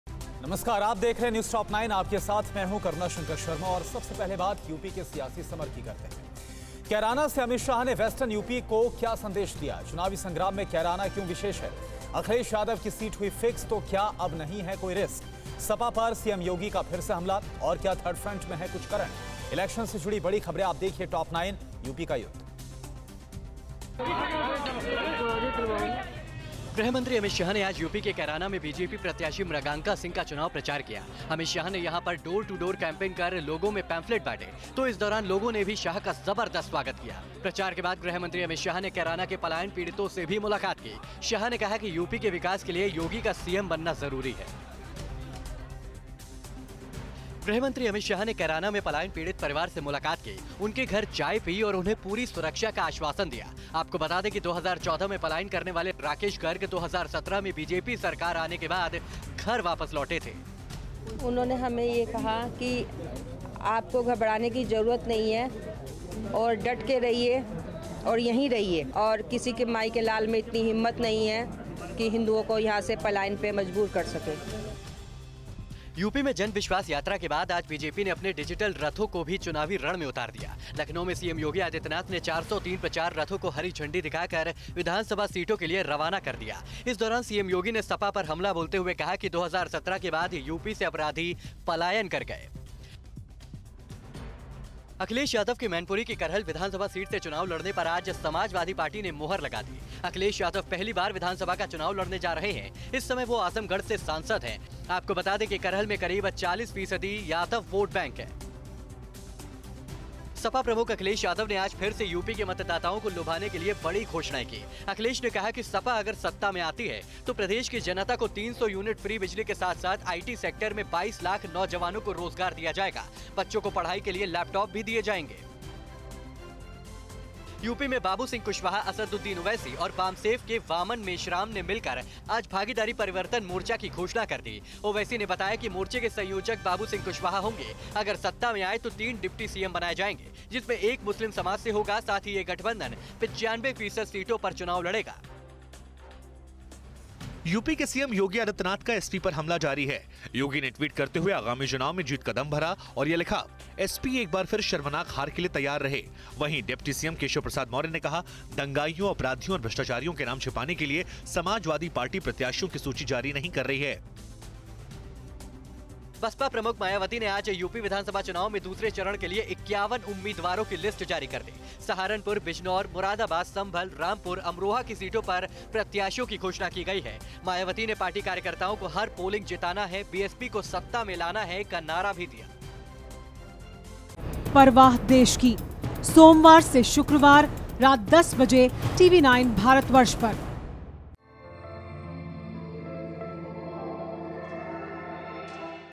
News Top 9 Election 2022 :UP फतह के लिए सभी पार्टियों ने प्रचार में झौंकी पूरी ताकत
गृहमंत्री अमित शाह ने आज यूपी के कैराना में बीजेपी प्रत्याशी मृगांका सिंह का चुनाव प्रचार किया.अमित शाह ने यहां डोर-टू-डोर कैंपेन कर लोगों में पैम्फ्लेट बांटे. इस दौरान लोगों ने भी शाह का जबरदस्त स्वागत किया.प्रचार के बाद गृहमंत्री ने कैराना के पलायन पीड़ितों से भी मुलाकात की.कैराना में 10 फरवरी को वोटिंग होगी

Credit